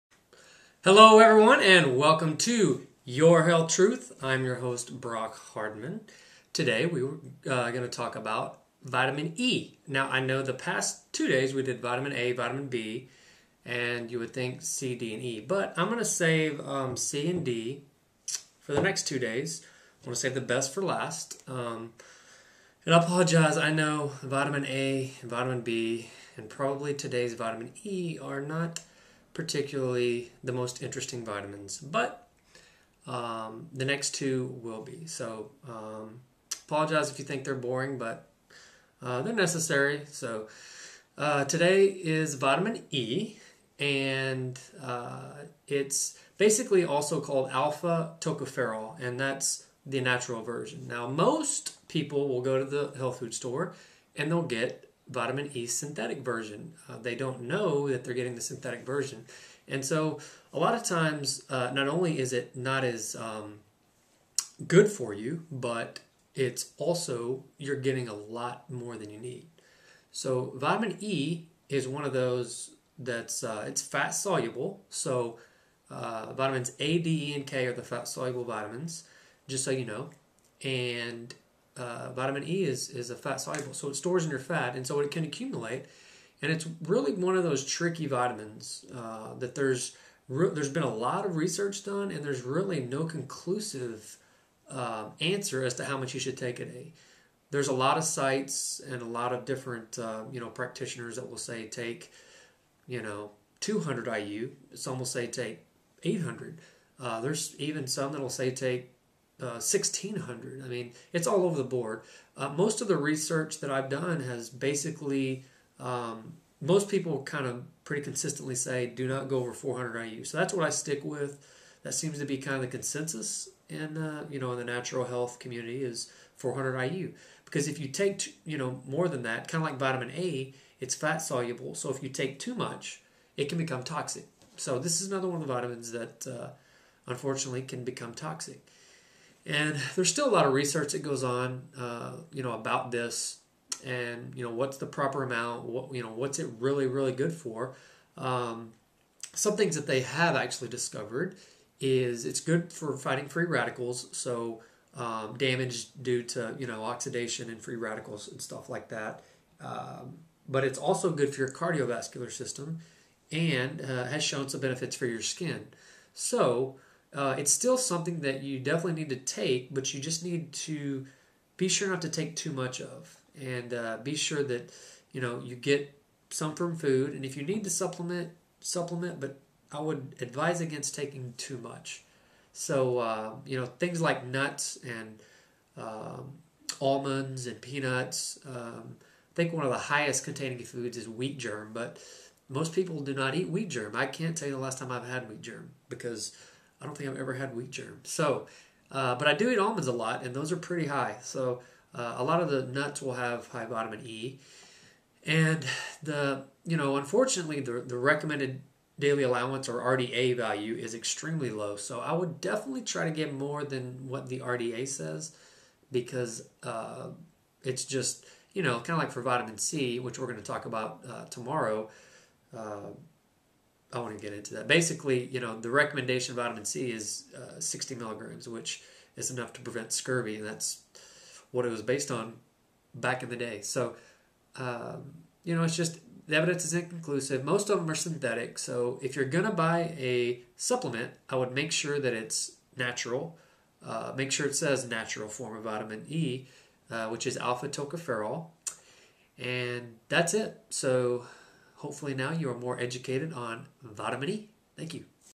Vitamin E
Learn about vitamin E and its role in the body with regards to antioxidant and cardiovascular ability.  Just be sure to consume the natural form of vitamin E when using in supplement form.